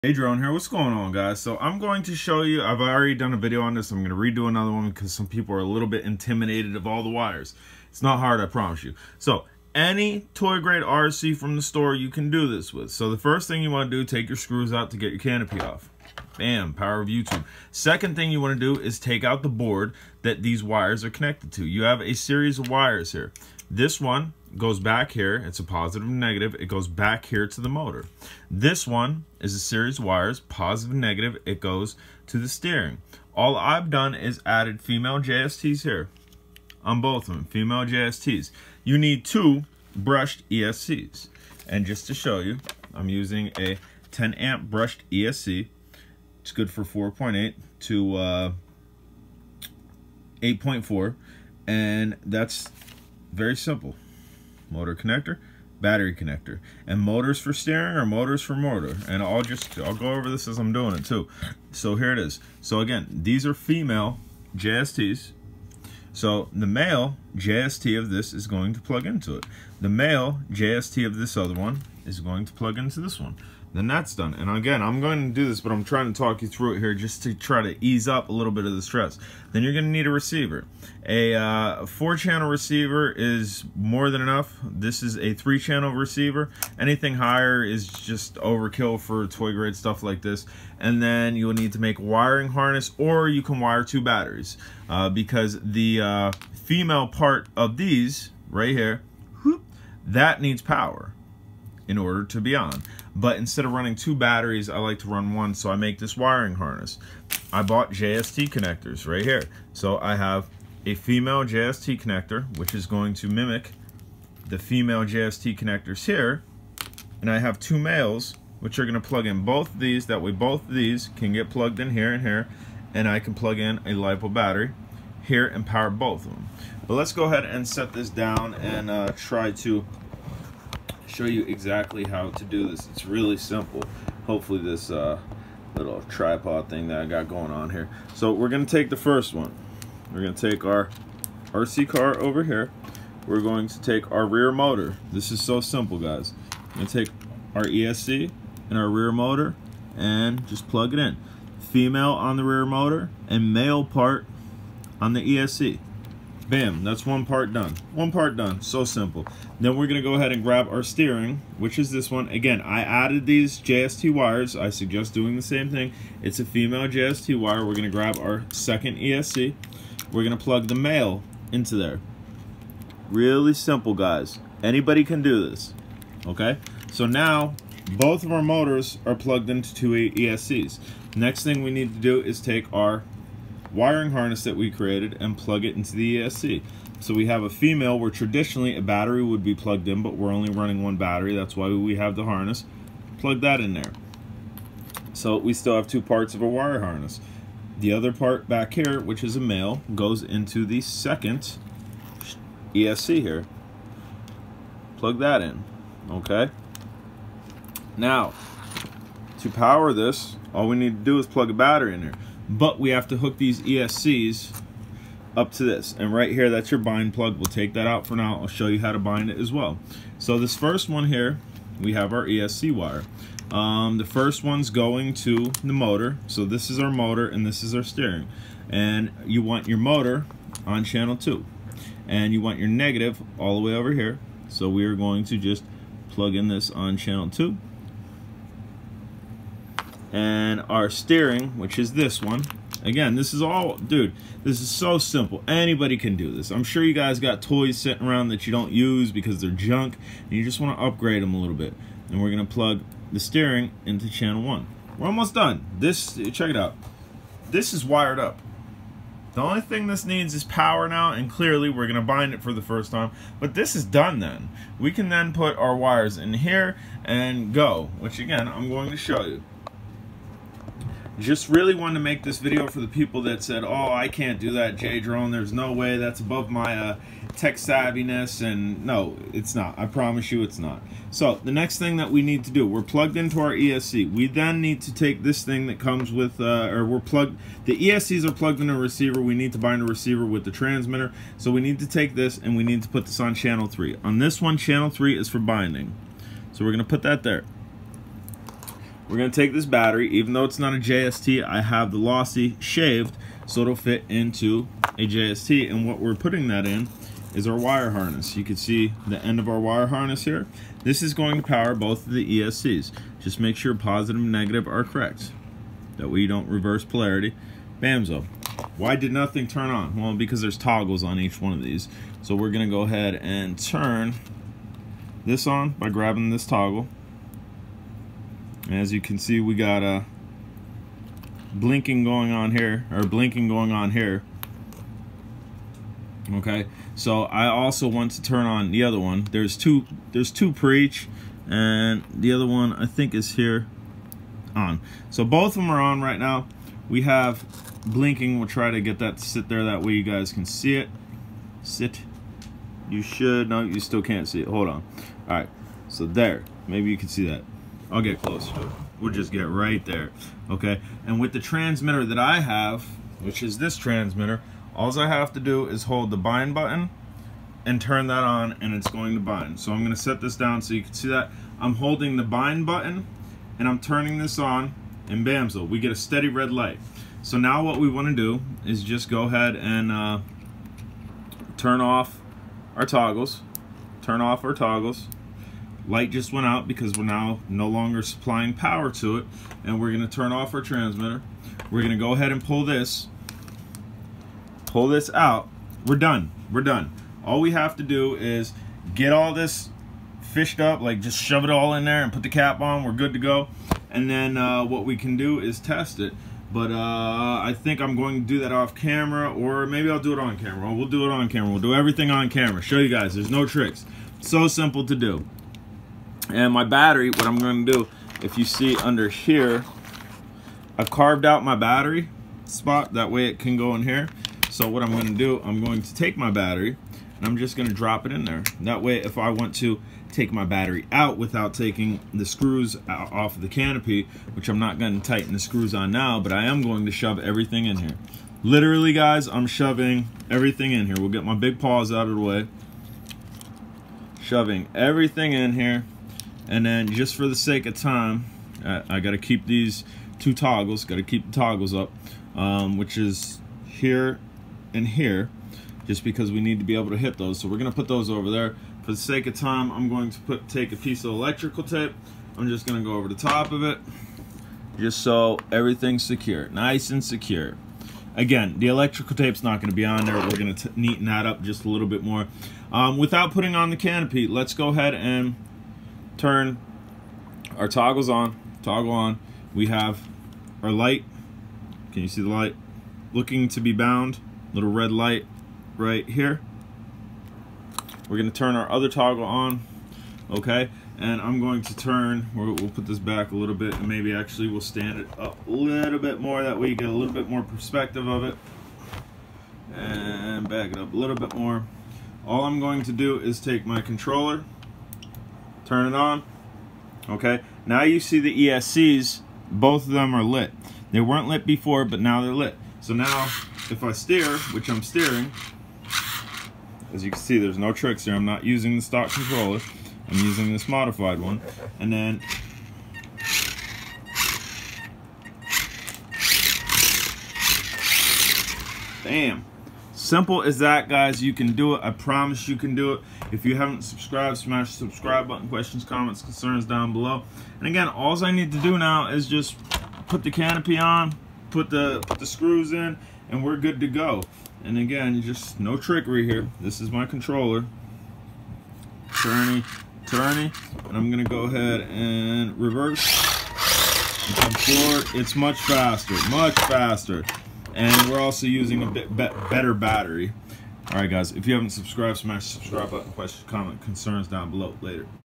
Upgrade any Toy RC Car Jada Hyperchargers SIMPLE DIY Flysky
Very simple to do PARTS USED
FLYSKY RECIEVER

ESC'S 2 of Hobbypower Rc 10a Brushed ESC

JST CONNECTORS

Flysky Controller